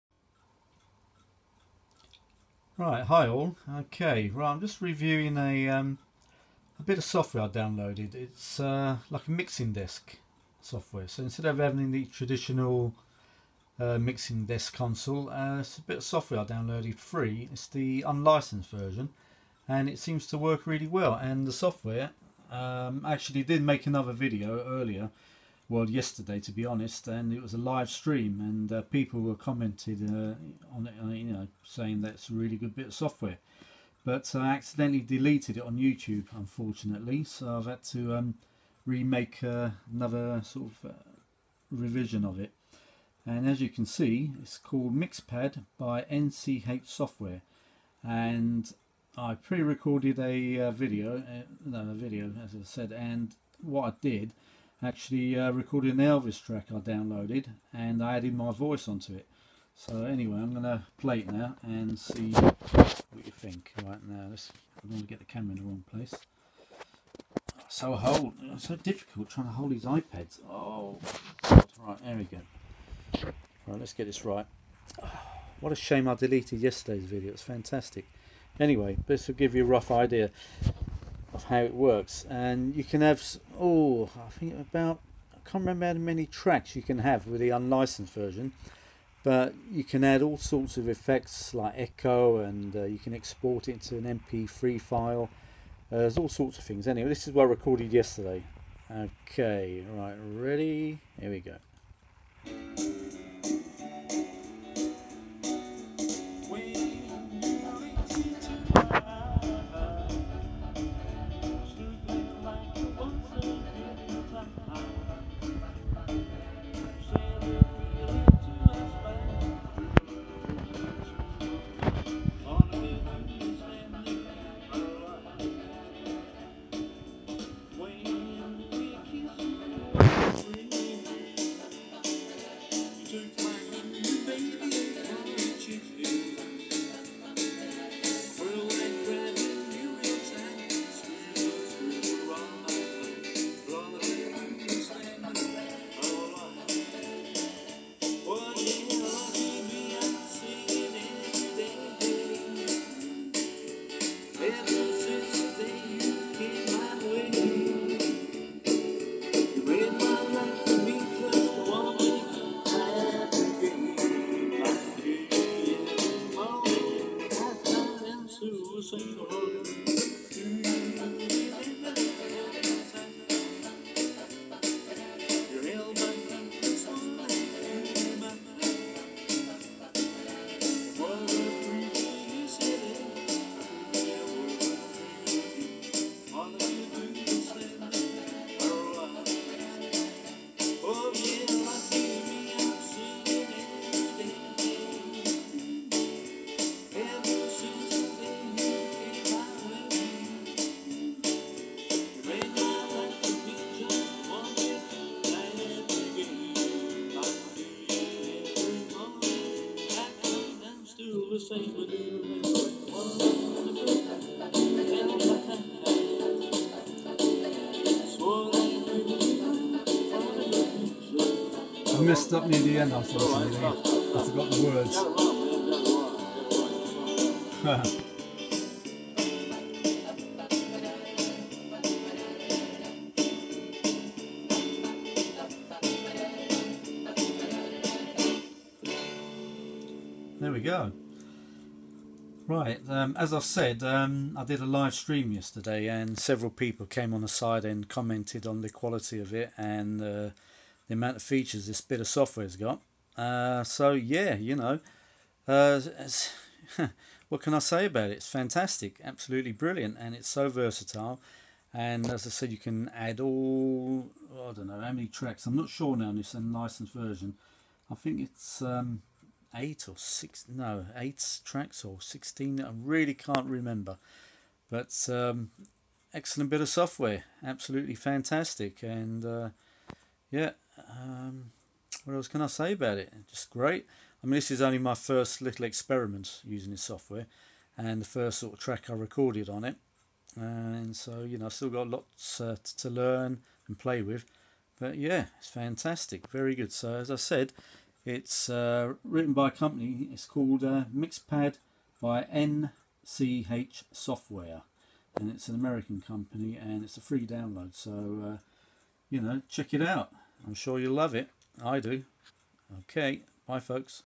Demo of Mixpad Multitrack Recording Software. By NCH Software.
A quick demo of a really great piece of mixing desk software by NCH Software, check it out it’s brilliant !